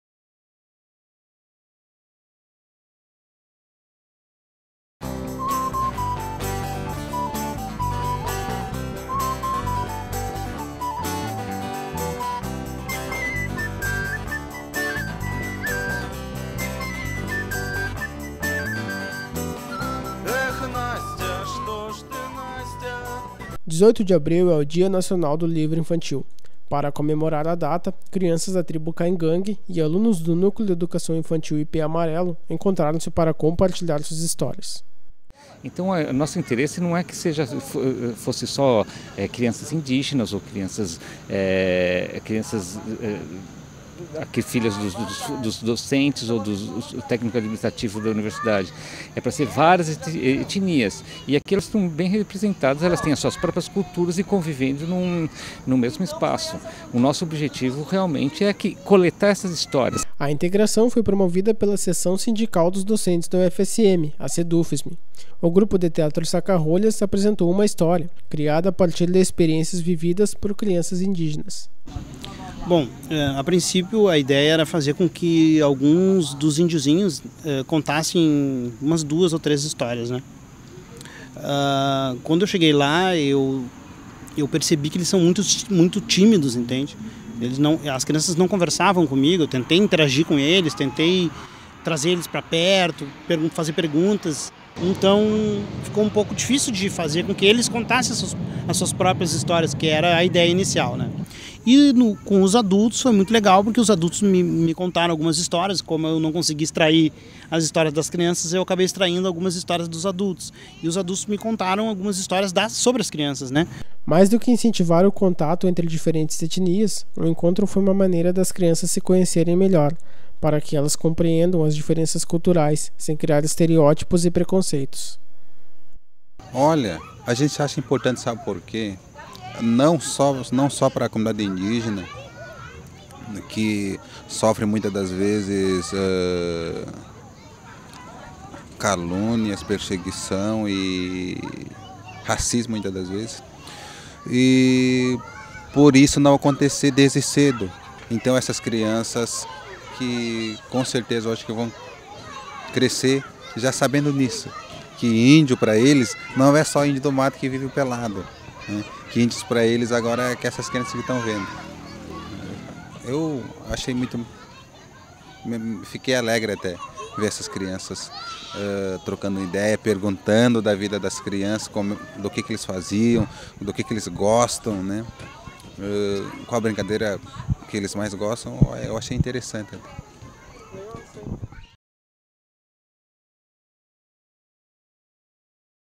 Integração Étnica e Cultural
Realização de um encontro entre crianças indígenas e não-indígenas, com o objetivo de ampliar a percepção cultural e étnica das crianças.